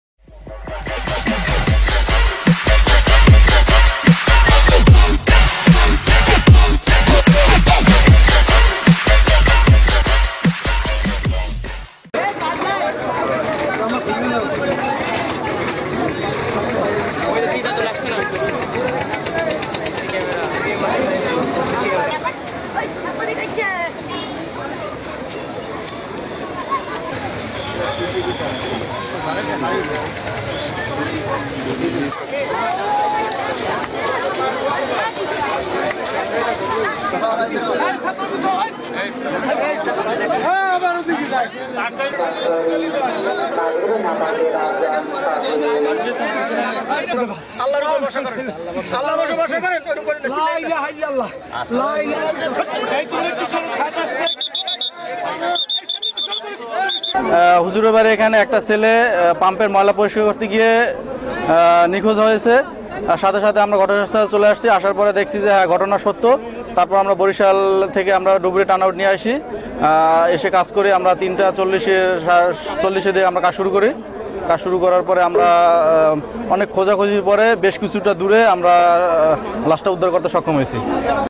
ঝালকাঠিতে নদীতে ডুবে যুবকের মৃত্যু - sheersha.tv
ঝালকাঠিতে মাদ্রাসার পানি উত্তোলনের পাইপ পরিস্কার করতে গিয়ে এক যুবক নদীতে ডুবে মারা গেছেন।-sheersha.tv= সট: ,মেহেদী হাসান
স্টেশন মাস্টার, ঝালকাঠি ফায়ার সার্ভিস ও সিভিল ডিফেন্স" বিস্তারিত জানতে ক্লিক করুন